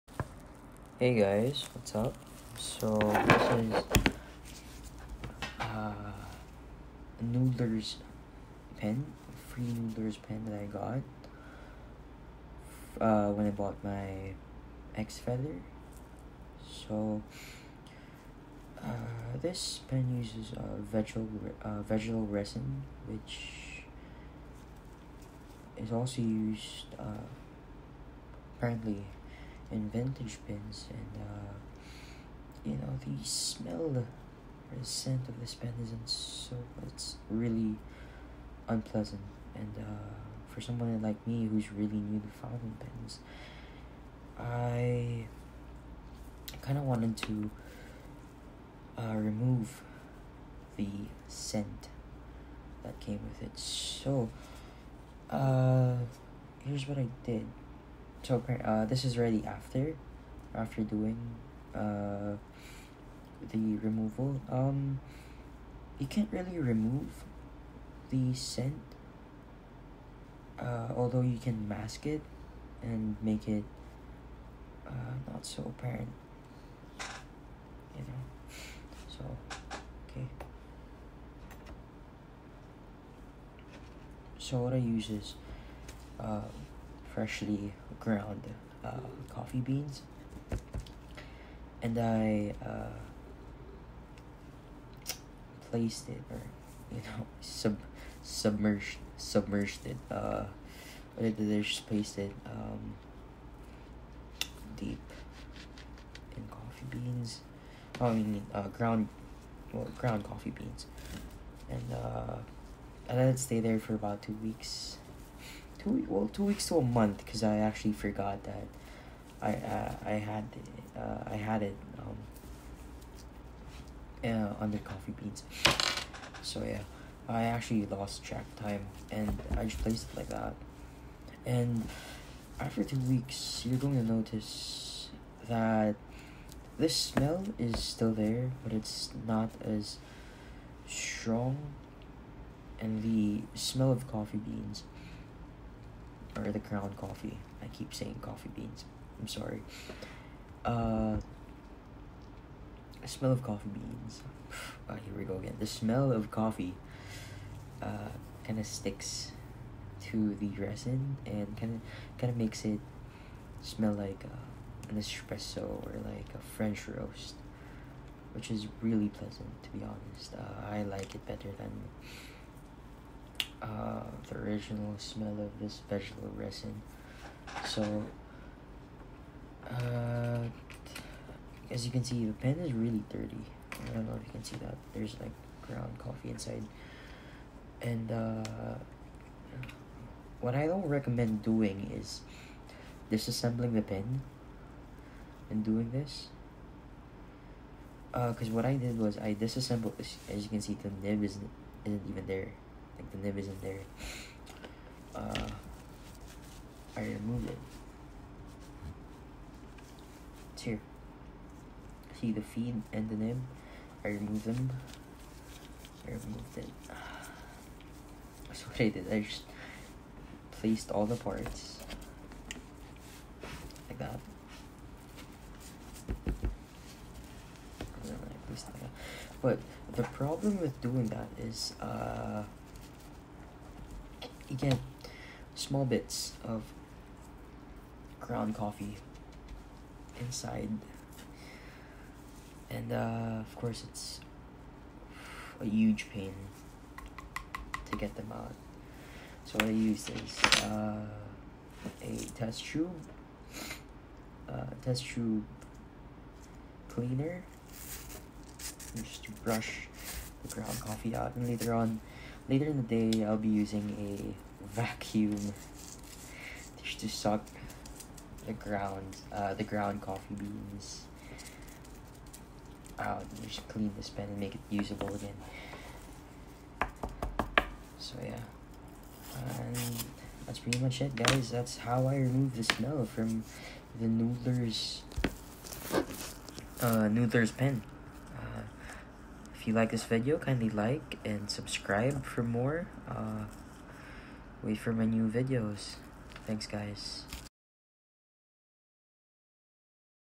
How to remove the noodler's pen smell
Welcome to my first YouTube video. I apologize for the delay and the quality of the audio. I'm saving to get better equipment and I will try to do better next time.